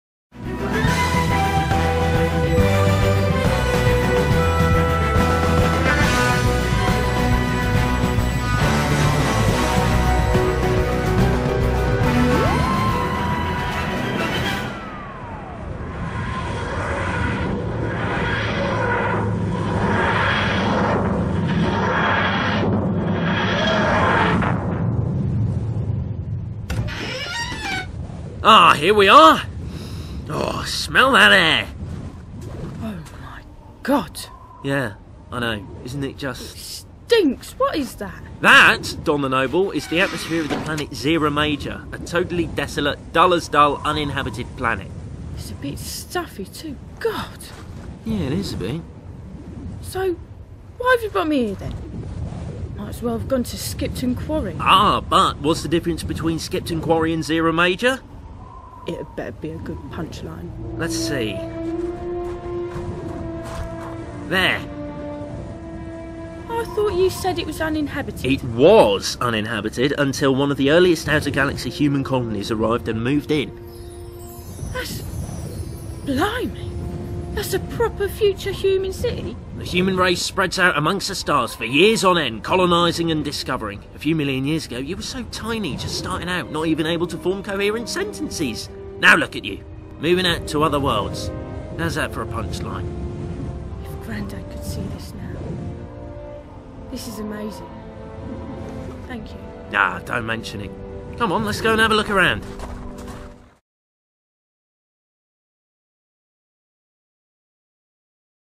PREVIEW CLIP - Doctor Who Action Figure Adventures: The New World
A sneak peak at the next Doctor Who AFA episode 'The New World' featuring the Tenth Doctor and Donna.

Coming soon...